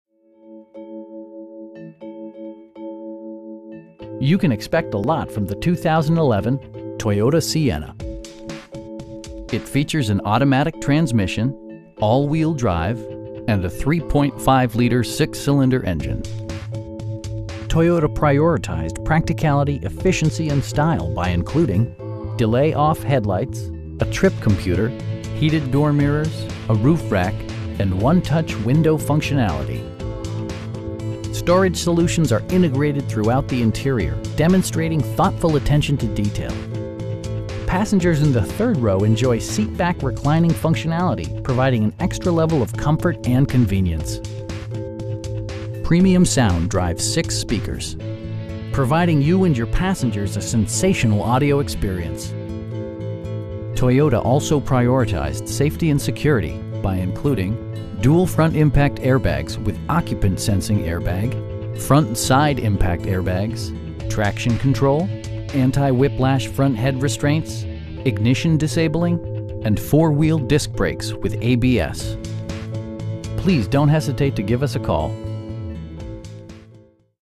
2011 Toyota Sienna in Toronto, ON M3J 2C7
You can expect a lot from the 2011 Toyota Sienna. It features an automatic transmission, all-wheel drive, and a 3.5 liter 6 cylinder engine. Toyota prioritized practicality, efficiency, and style by including: delay-off headlights, a trip computer, telescoping steering wheel, heated door mirrors, power windows, a roof rack, and 1-touch window functionality. Storage solutions are integrated throughout the interior, demonstrating thoughtful attention to detail. Passengers in the third row enjoy seat back reclining functionality, providing an extra level of comfort and convenience. Premium sound drives 6 speakers, providing you and your passengers a sensational audio experience. Toyota also prioritized safety and security by including: dual front impact airbags with occupant sensing airbag, front side impact airbags, traction control, anti-whiplash front head restraints, ignition disabling, and 4 wheel disc brakes with ABS. Brake assist technology provides extra pressure when applying the brakes. We pride ourselves in consistently exceeding our customer's expectations. Please don't hesitate to give us a call.